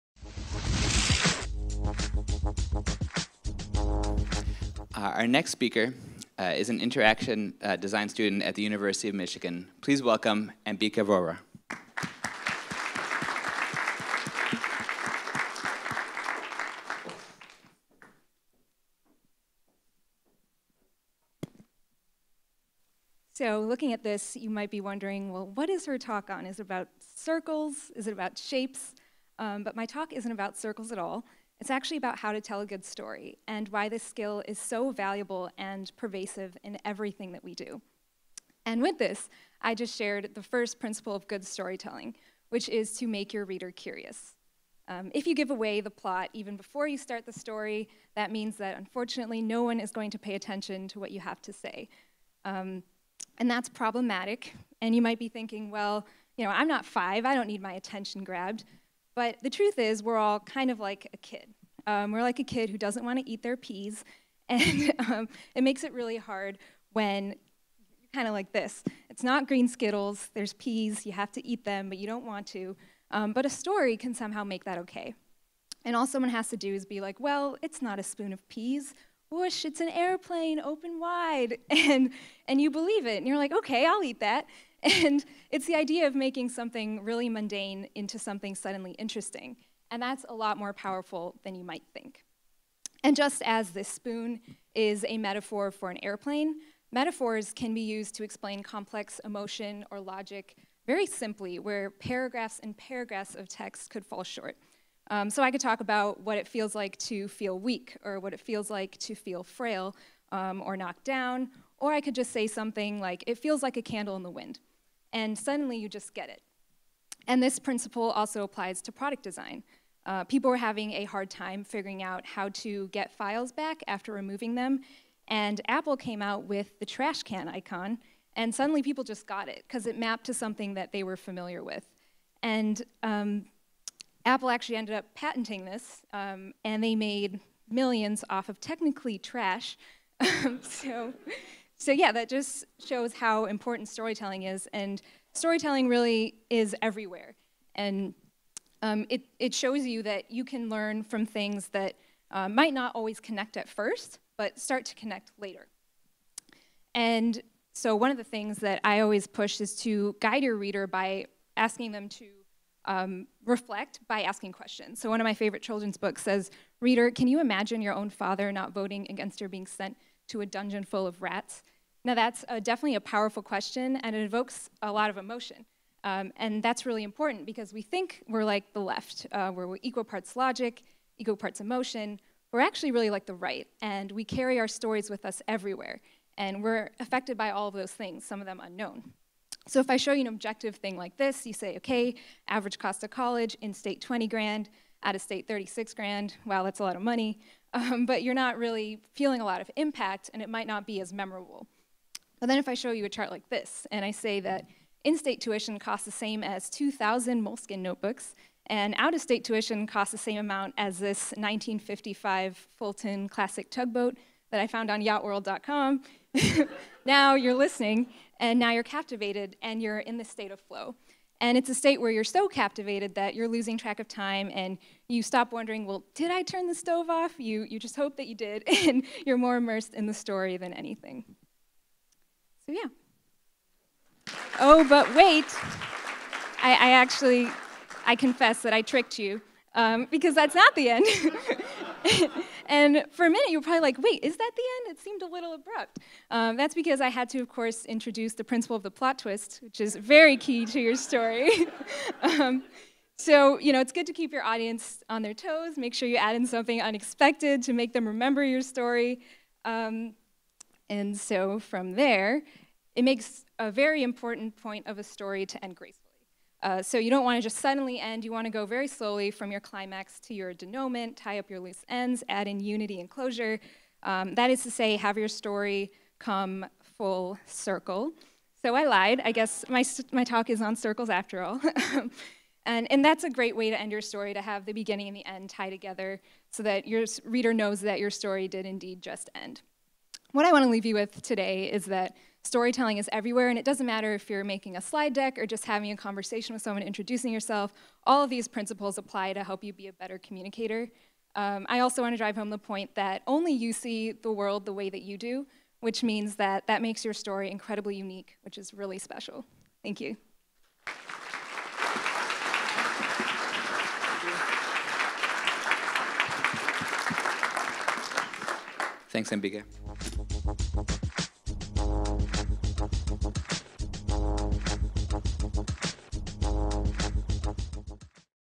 20181102-IgniteA2_11-talk#14-Ambika Vohra - Art of the Arc
Ambika Vohra
"Art of the Arc"

Talk #14 - Ignite Ann Arbor #11
7-9pm, Friday, November 2, 2018
Ann Arbor District Library-Downtown-Lobbytorium